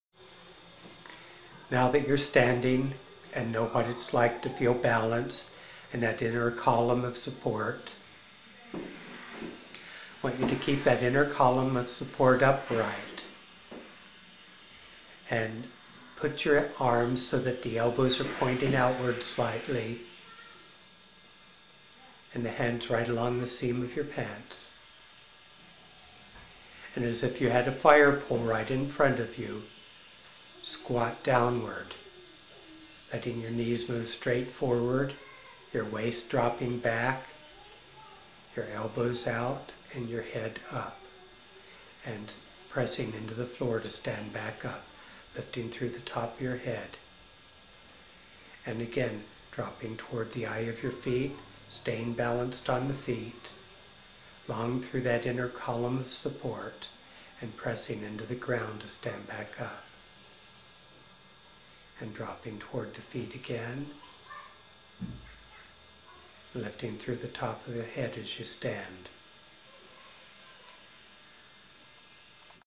Four Directions
The four dirctions of movement as taught in early Rolfing, are elbows out, knees forward, crown up, and waist back.  This is an exercise constantly taught and performed by Peter Melchior.